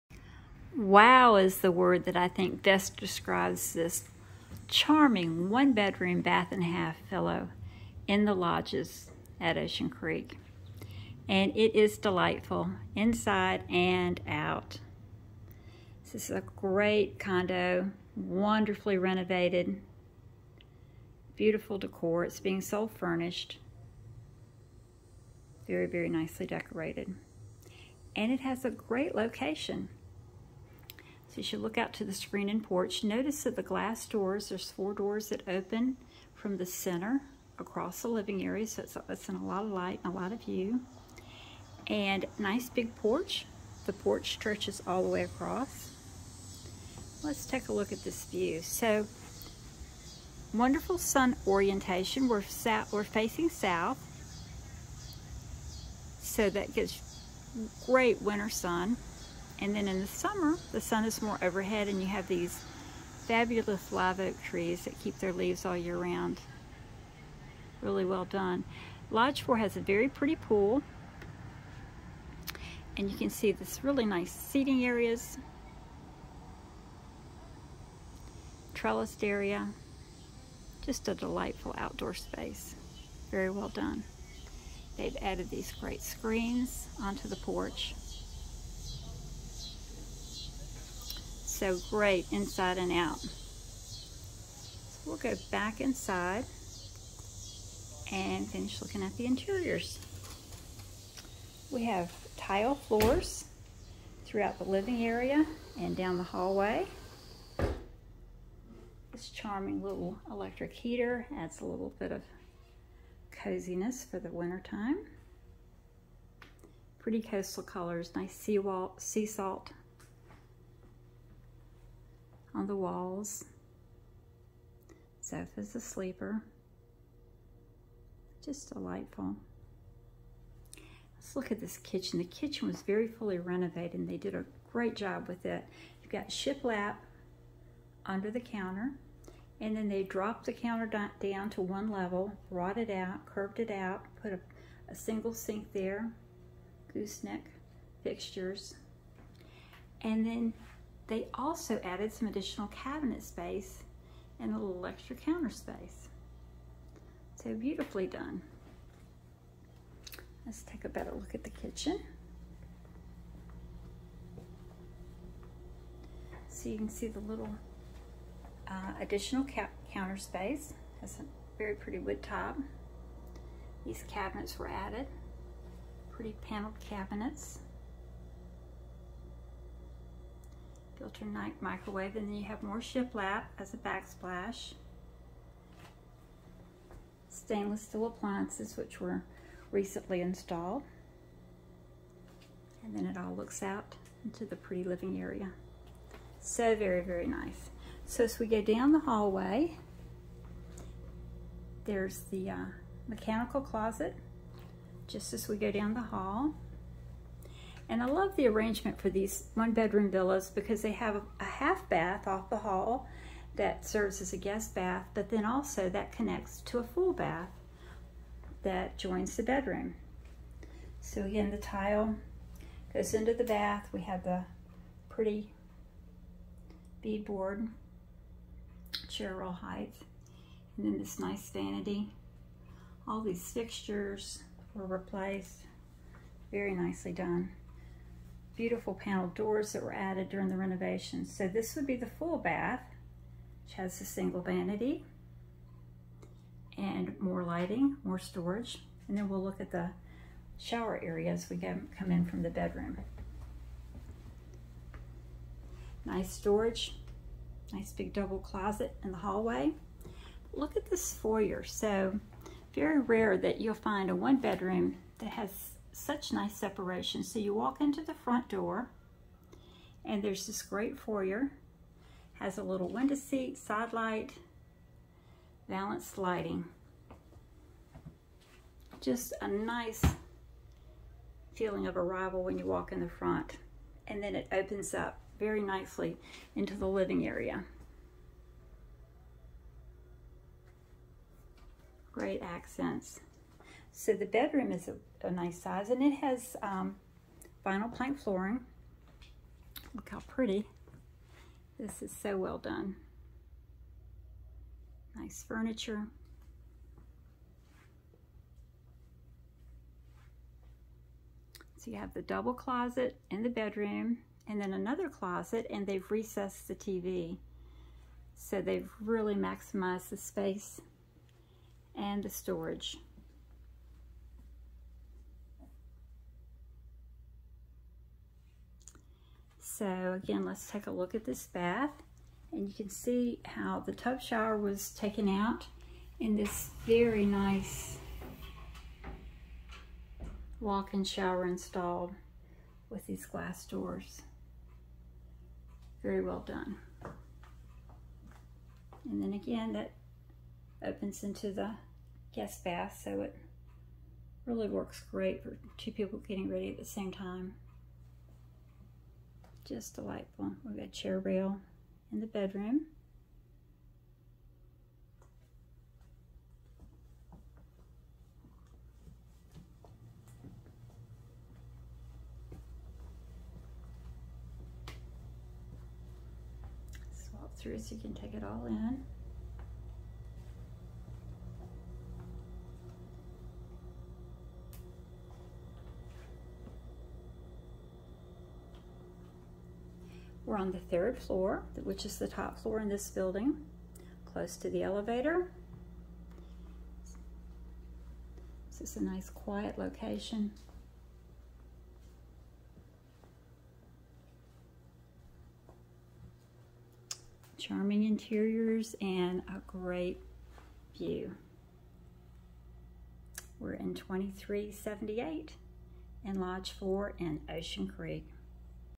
Ocean Creek 2378 Lodge 4 - Sold
This is a beautiful one bedroom, full plus half bath condo in Ocean Creek.